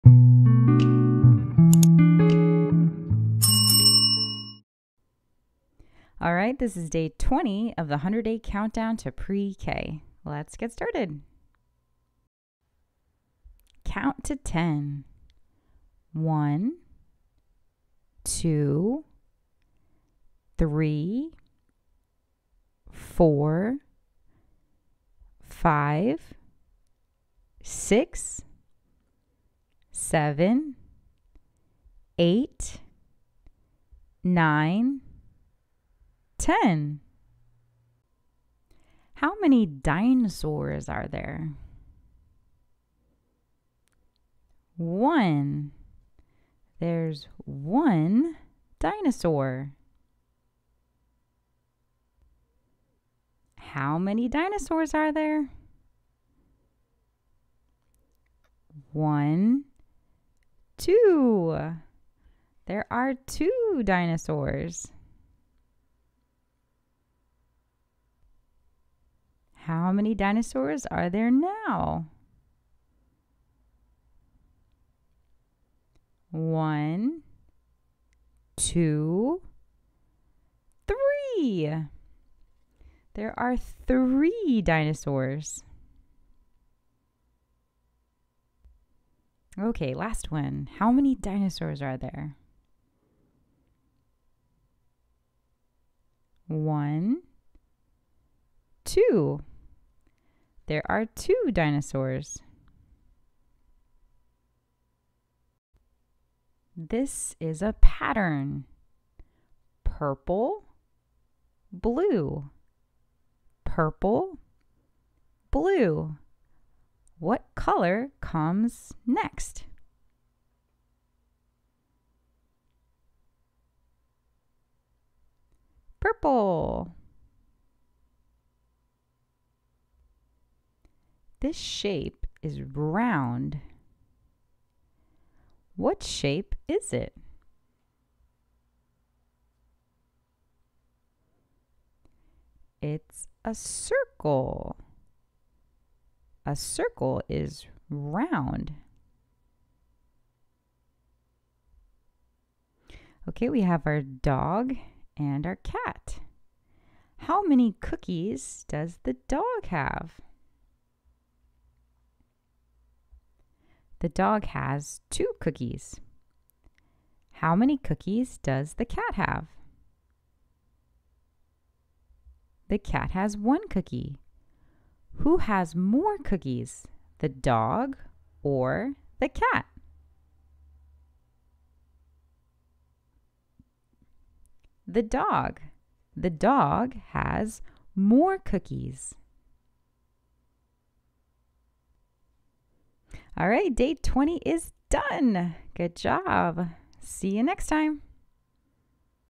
Day 20: Math 100-Day Countdown to Pre-K
Florida Early Learning & Developmental Standards
Mathematical Thinking (ages 3-4)
A. Number Sense
1. Subitizes (immediately recognizes without counting) the number of objects in a set of four objects.
2. Counts and identifies the number sequence “1 to 10”.
3. Begins to demonstrate one-to-one correspondence up to 10 during daily routines.
4. Identifies the last number spoken tells “how many” up to five (cardinality).
5. Counts sets constructed by the teacher to five and beyond.
C. Patterns
1. Notices a pattern with a missing object and completes the pattern by filling in the missing object.
D. Geometry
1. Recognizes and names typical shapes (circle, square, triangle).

Clipart from MyCuteGraphics.com